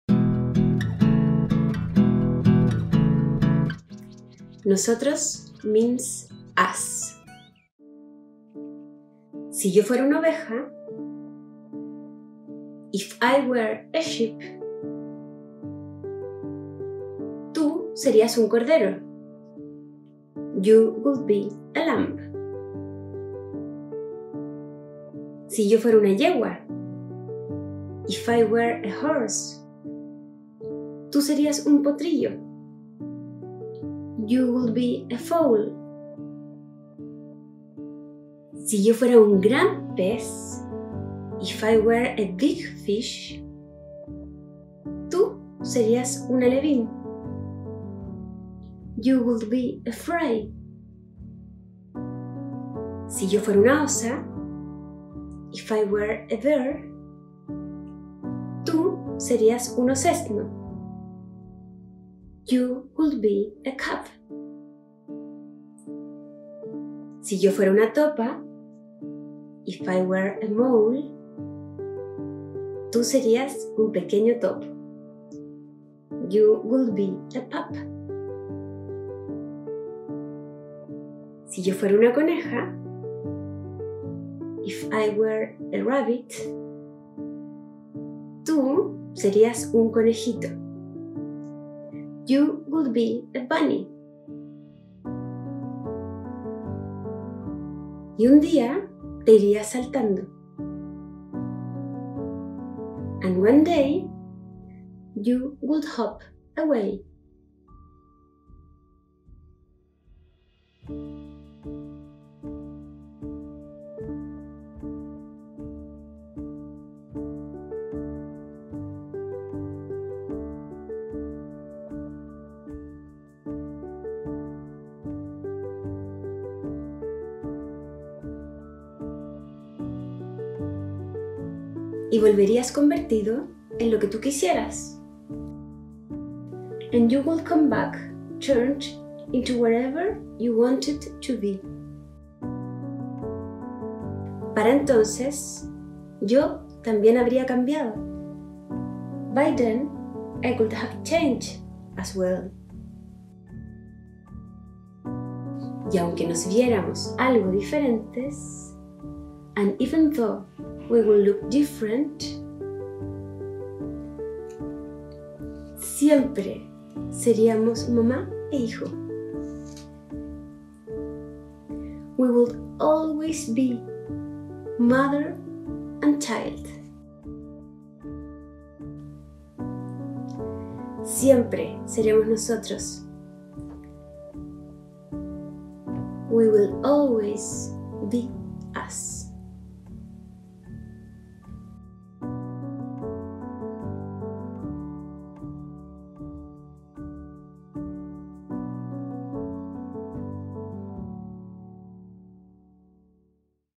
Nosotros Means Us (Bilingual) - Read Aloud with Author Paloma Valdivia | Brightly Storytime Together
A moving bilingual ode to the unshakeable bond between a parent and child.

Music used with permission from DeWolfe Music Library.